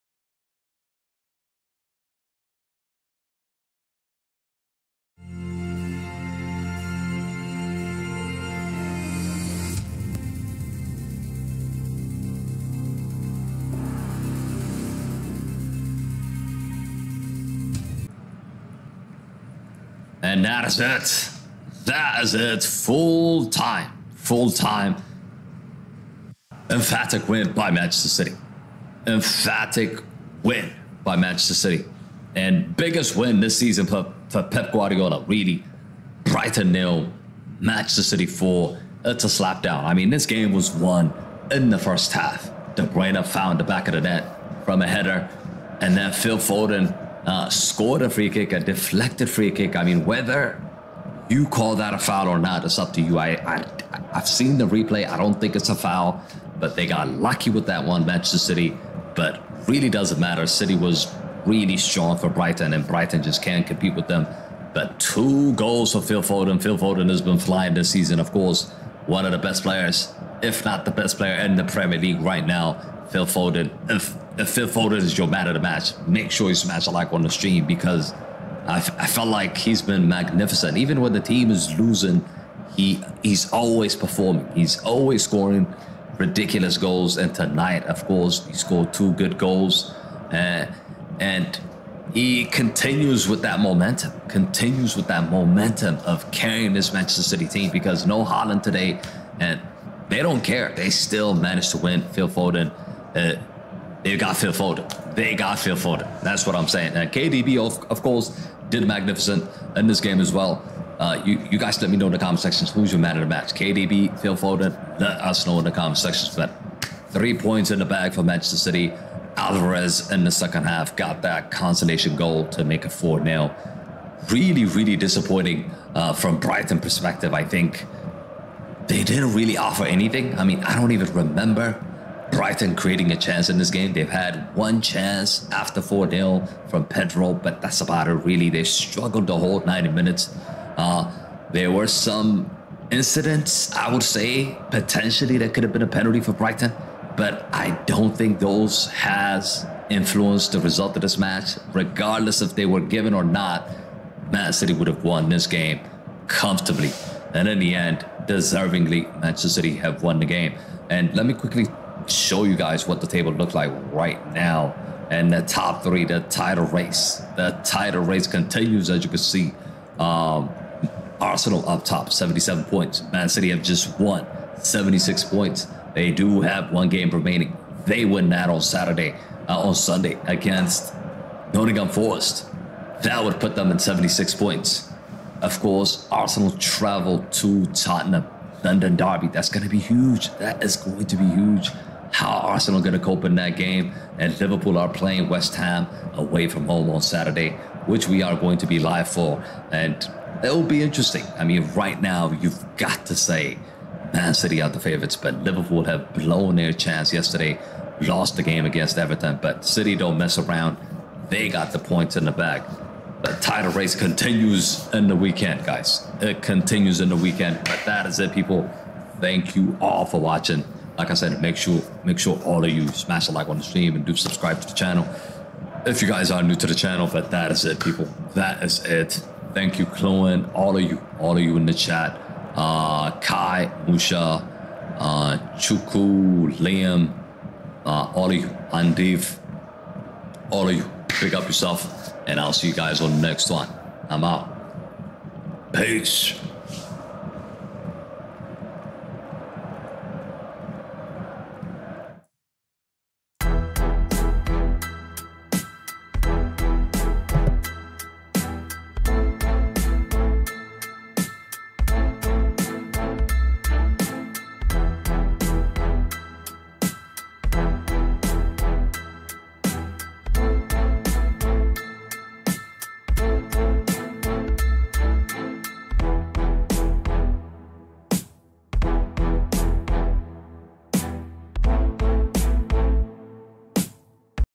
PHIL FODEN DOUBLE! BRIGHTON 0-4 MANCHESTER CITY
Brighton 0-4 Manchester City: Phil Foden double closes gap on Premier League leaders Arsenal to one point! Report and free match highlights as Manchester City close gap on Premier League leaders Arsenal to one point with 4-0 victory at Brighton; Kevin De Bruyne opens scoring with superb diving header; Phil Foden nets first-half double; Julian Alvarez adds fourth in second half📱Download SofaScore, the best football app for stats, scores & more!